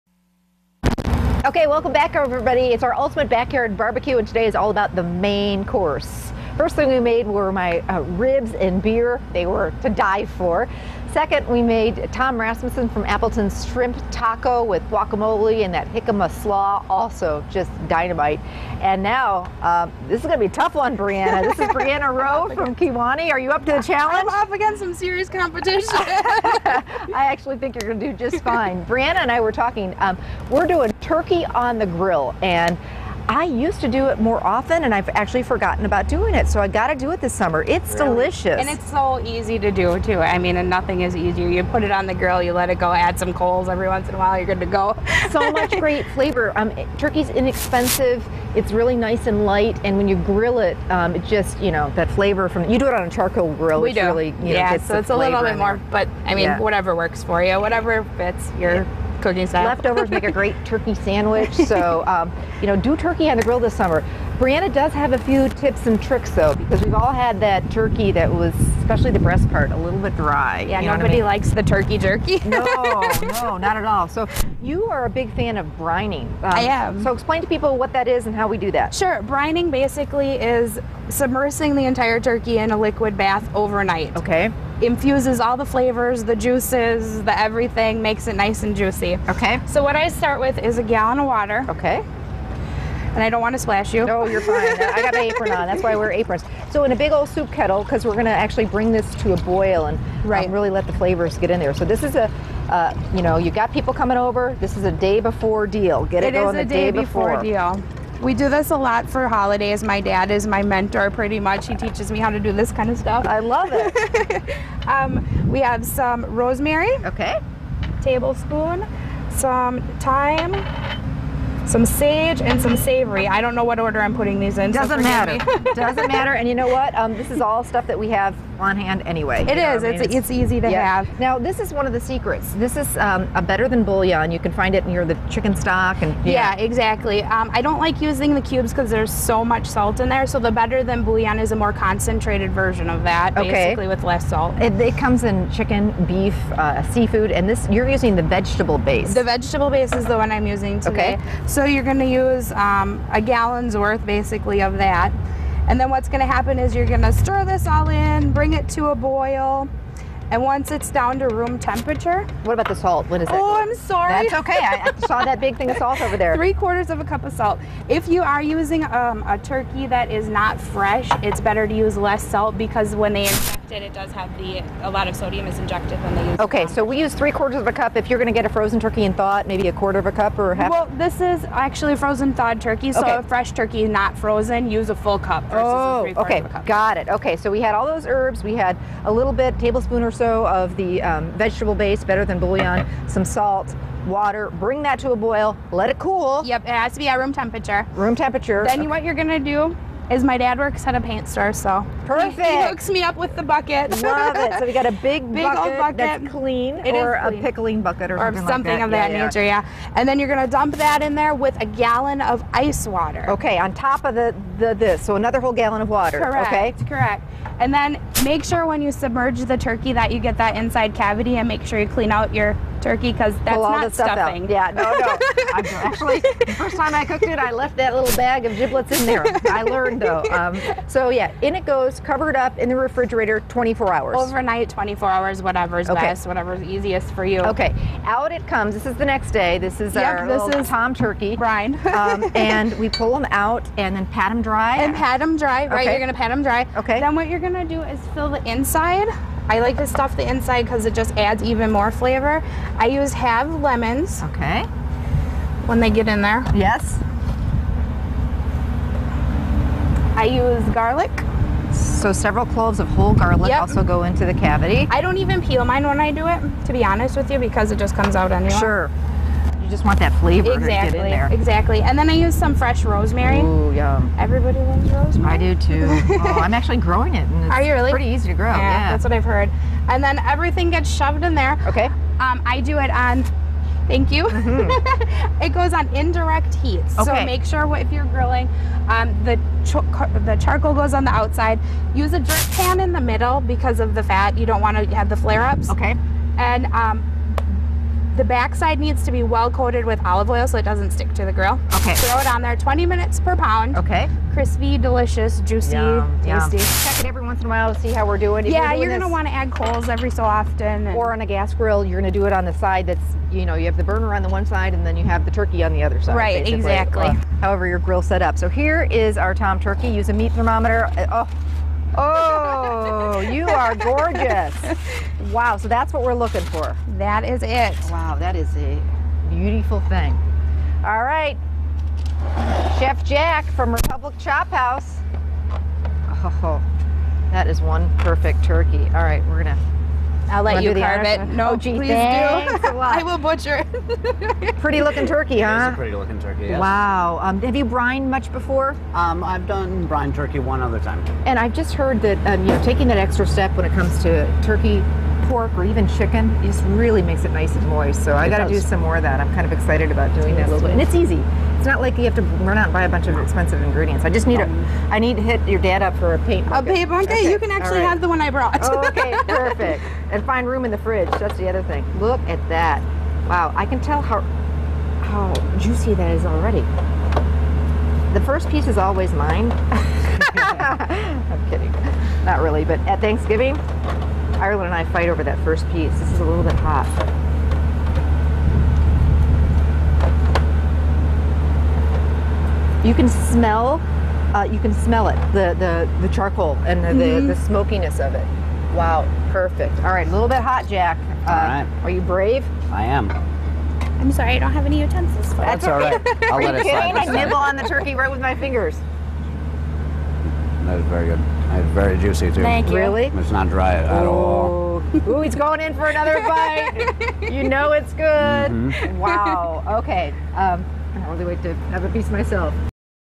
brined turkey
viewer recipe from turkey on the grill.